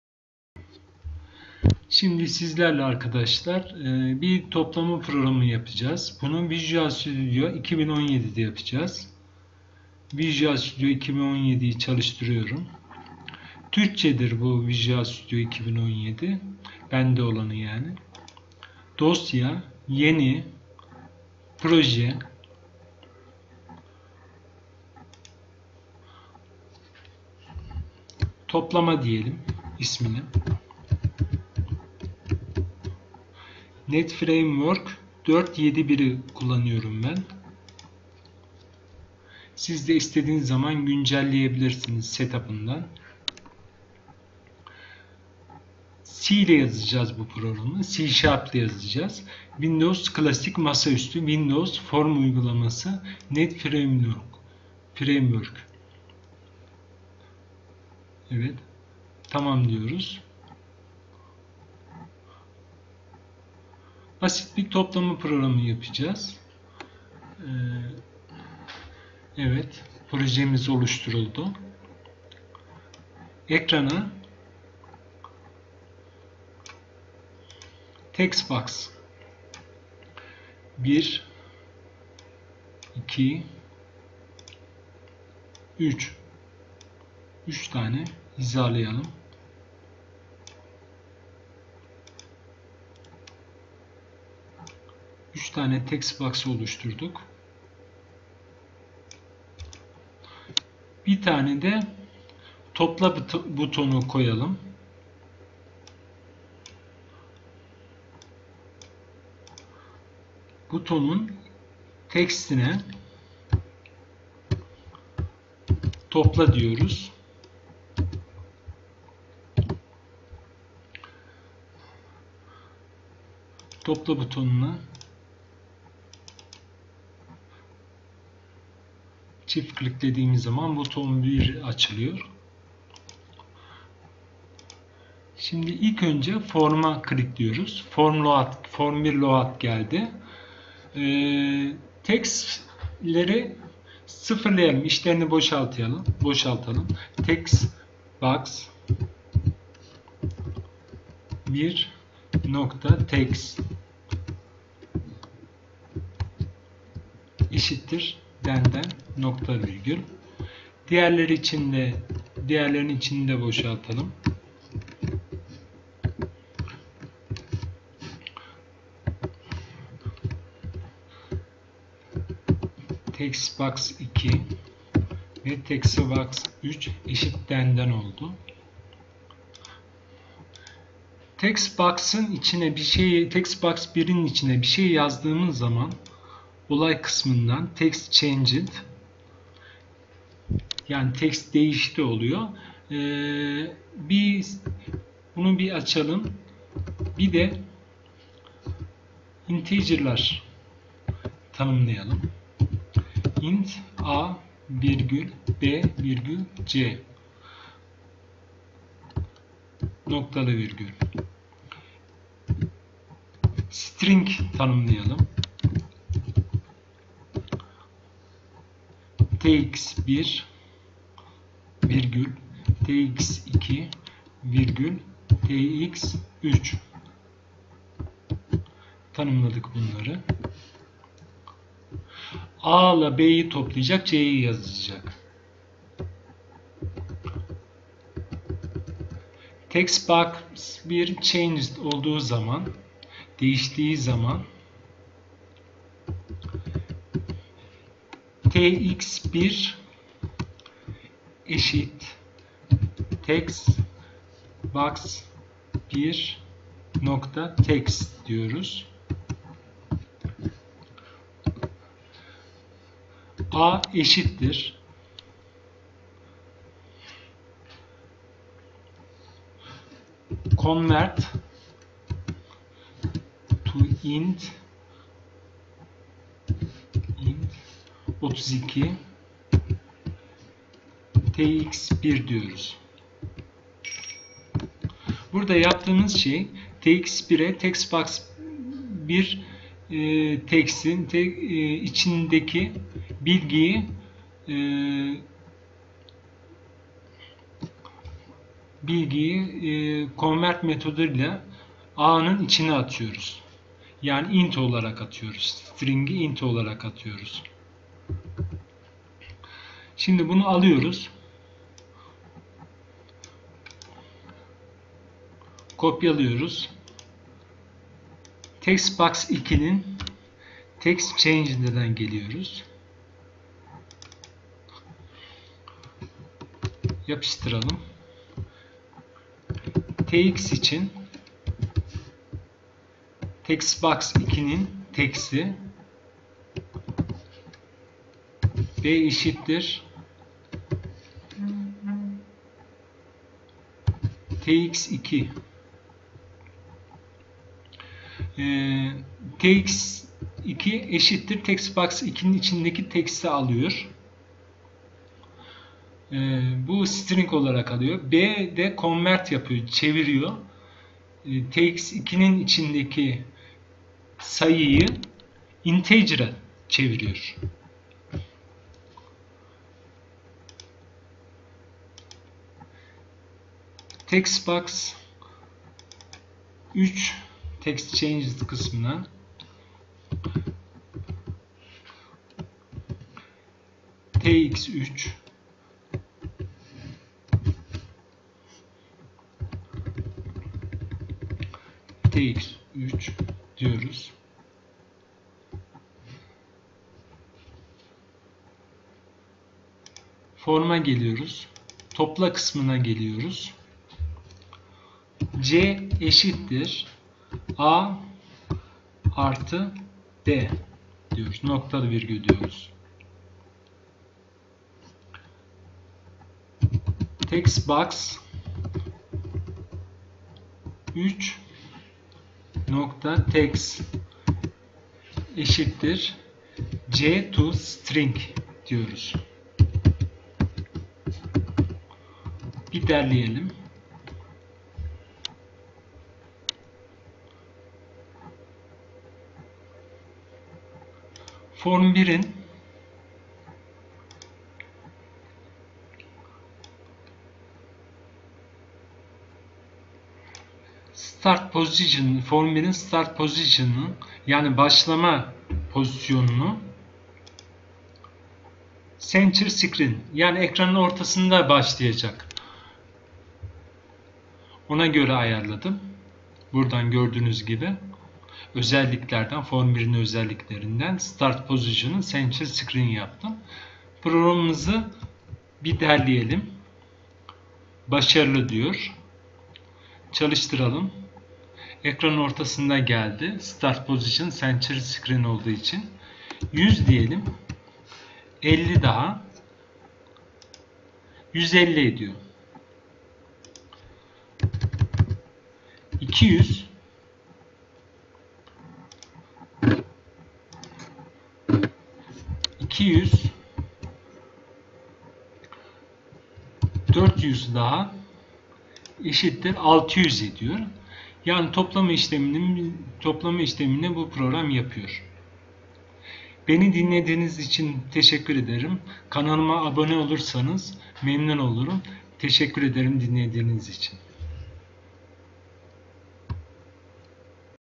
Visual Studio 2017 Türkçe C# ile Toplama programı yazma bölüm 1/2 | C# Dersleri
Visual Studio 2017 programının Türkçesiyle  C#  diliyle Toplama programını yazmayı gösteren iki videodan birincisi.
Birinci partın Kaynak kodu indirmek için linkine klikleyin.

Toplama programımın bu birinci partı ikinci parttan programımın tamamlanmış hali var, oradan kodlanışını izleyip indirebilirsiniz.